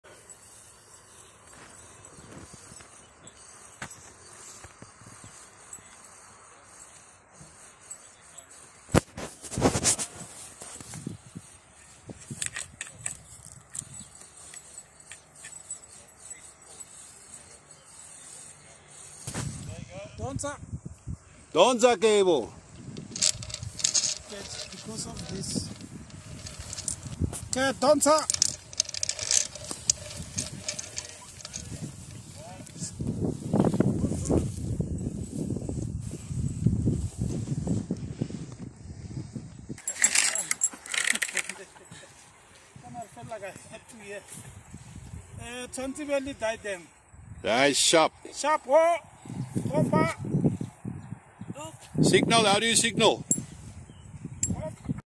How to string Aerial cable on poles (Ground level Demonstration
On the OSP Projects (FOA) pactical - a close view on how to haul fibre cable on overhead poles in a process to avoid any obstacles that may affect the stringing process.